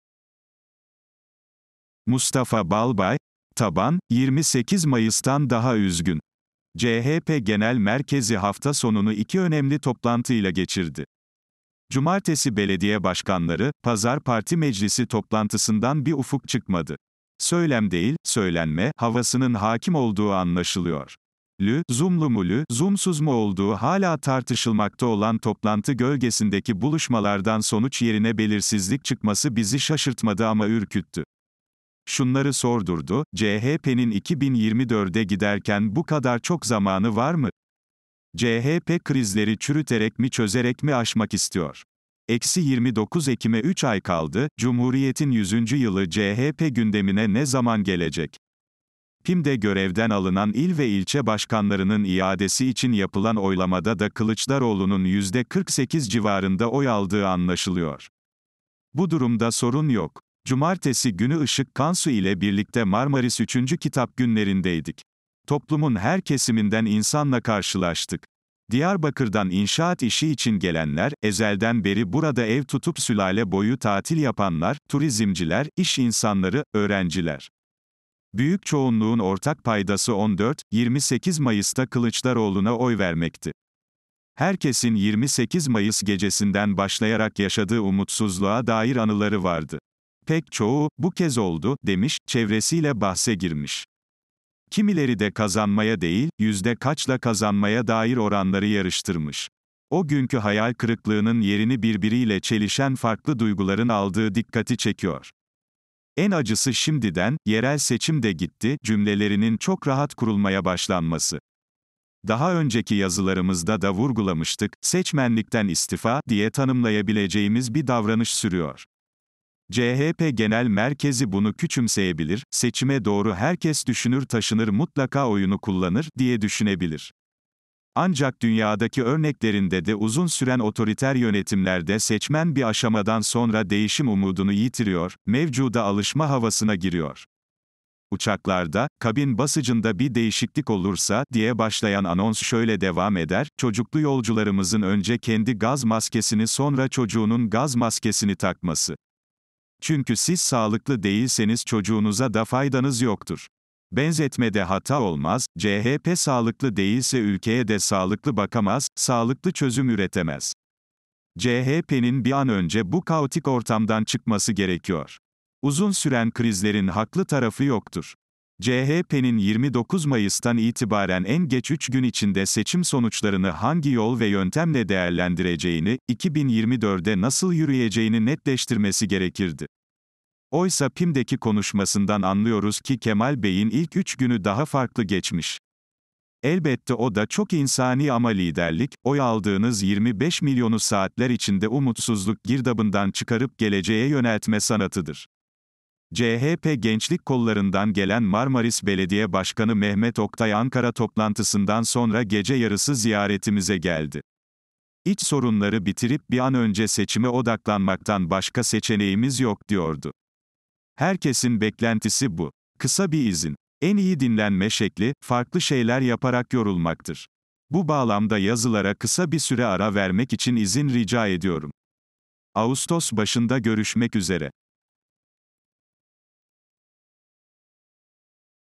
MUSTAFA BALBAY: TABAN, 28 MAYIS’TAN DAHA ÜZGÜN! - SESLİ KÖŞE YAZISI
Mustafa Balbay - cumhuriyet.com.tr - Taban, 28 Mayıs’tan daha üzgün! - 25 Temmuz 2023 tarihli yazısının seslendirmesi

"CHP Genel Merkezi hafta sonunu iki önemli toplantıyla geçirdi. Cumartesi belediye başkanları, pazar parti meclisi (PM) toplantısından bir ufuk çıkmadı.

“Söylem”değil, “söylenme” havasının hâkim olduğu anlaşılıyor.

Lü’zoom’lu mu lü’zoom’suz mu olduğu hâlâ tartışılmakta olan toplantı gölgesindeki buluşmalardan sonuç yerine belirsizlik çıkması bizi şaşırtmadı ama ürküttü! Şunları sordurdu:

-CHP’nin 2024’e giderken bu kadar çok zamanı var mı?

-CHP krizleri çürüterek mi çözerek mi aşmak istiyor?